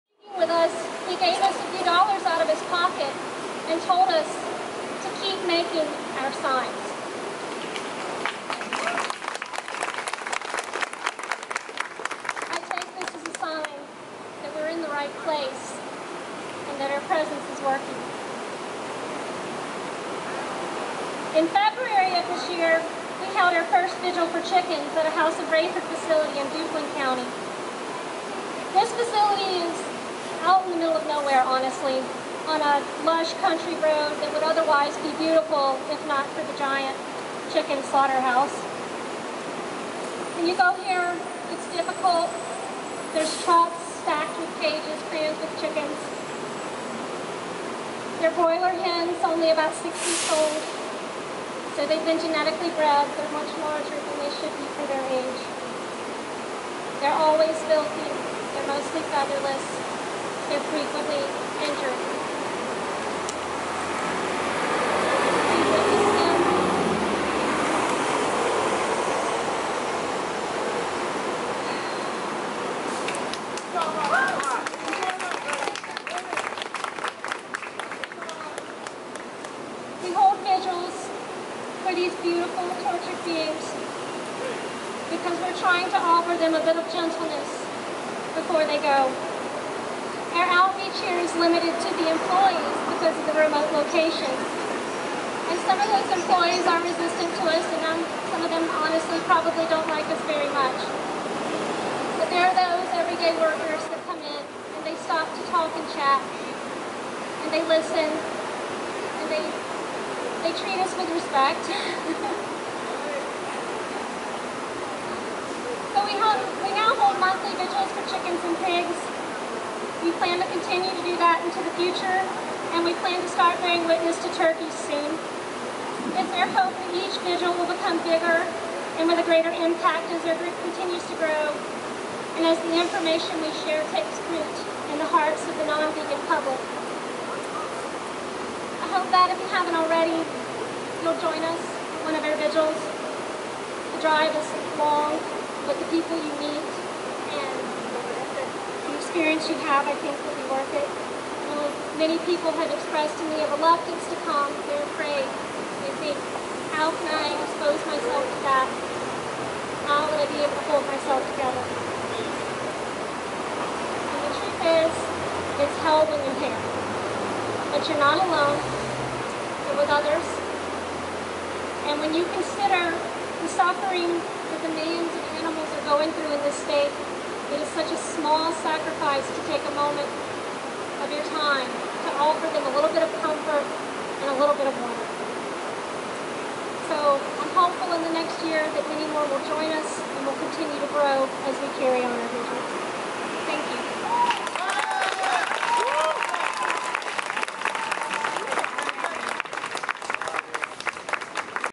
Tracy Curtis of North Carolina Farmed Animal Save
Tracy Curtis gives a talk at Charlotte's first National Animal Rights Day on June 5th, 2016 about the progress of North Carolina Farmed Animal Save